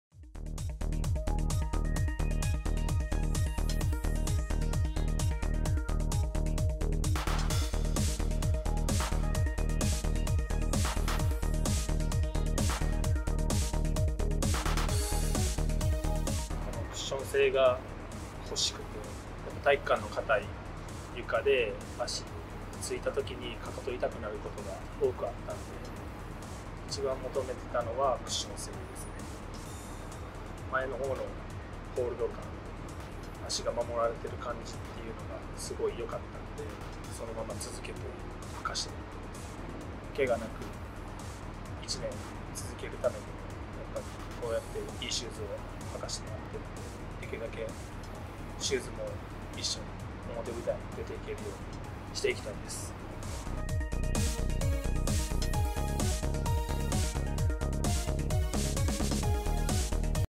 ATHLETA（アスレタ）｜11011 O-Rei Futsal Rodrigo｜Fリーガー 日根野谷建
11011  O-Rei Futsal Rodrigo 19AW NEW Color
Fリーガー  日根野谷建選手（ペスカドーラ町田）が語る O-Rei Futsal Rodrigoとは。